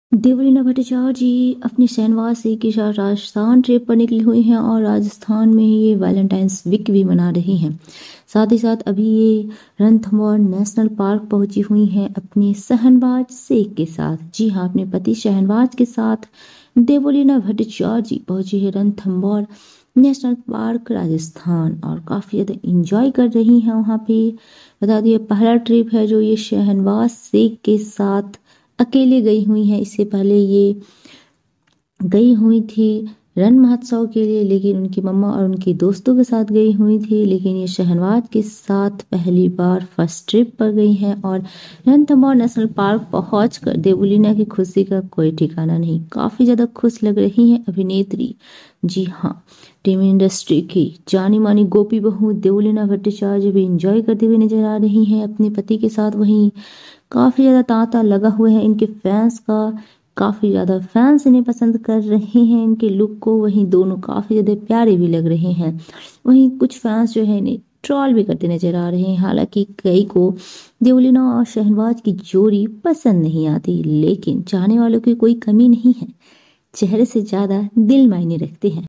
Devoleena bhattacharjee shahnawaz shaikh enjoy in Ranthambore national park
Devoleena bhattacharjee and shahnawaz shaikh arrived in Rantambore National park